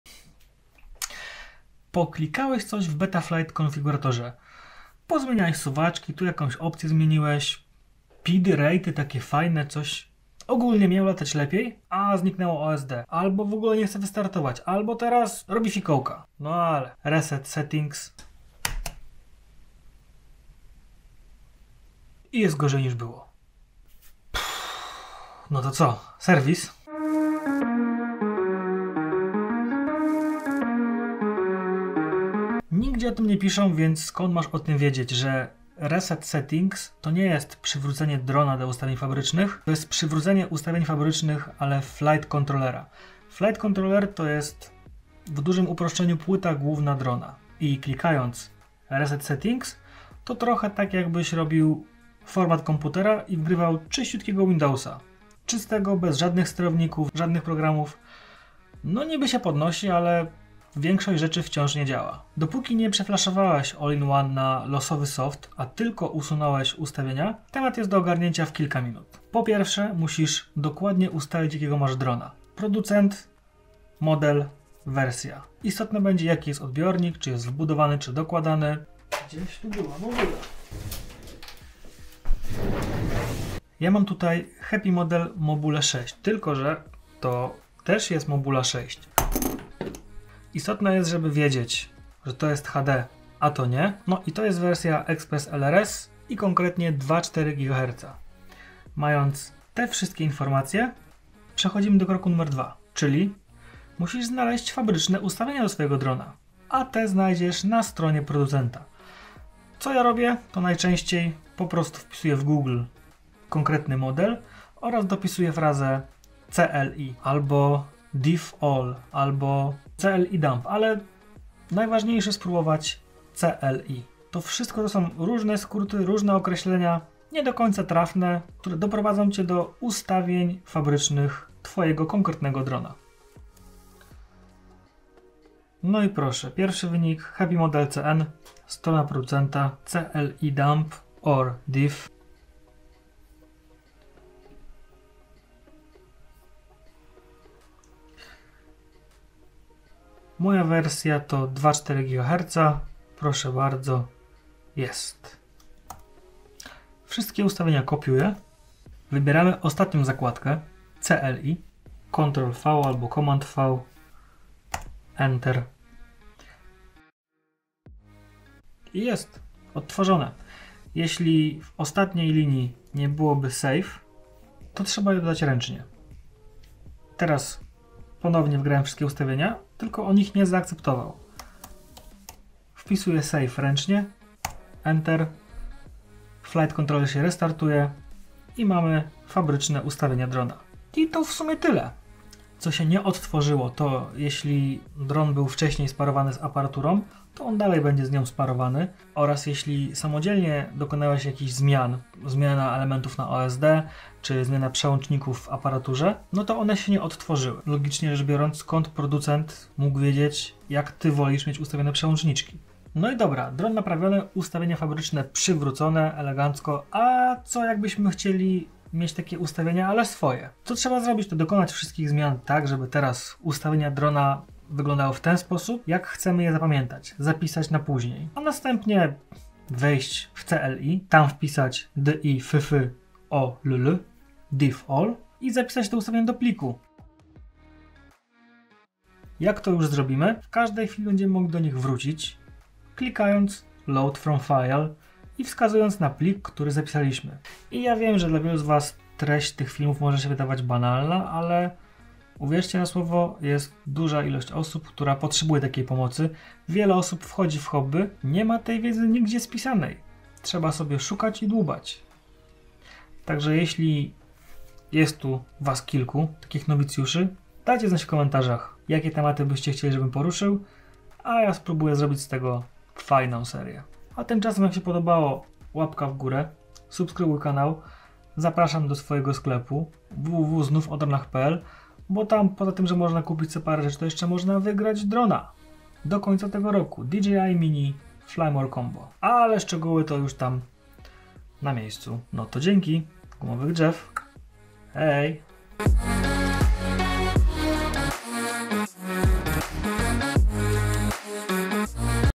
Przywrócenie ustawień fabrycznych w BETAFLIGHT. Reset drona FPV. DIFF ALL! reset settings cli
Problem dla starych wyjadaczy banalny, dla nowych w hobby często stanowi problem nie do przejścia. ZRESETOWAŁEM SOBIE DRONA, CO TERAZ?

▼ Drony, którymi latam i polecam!

▼ Najłatwiej dobić się do mnie poprzez email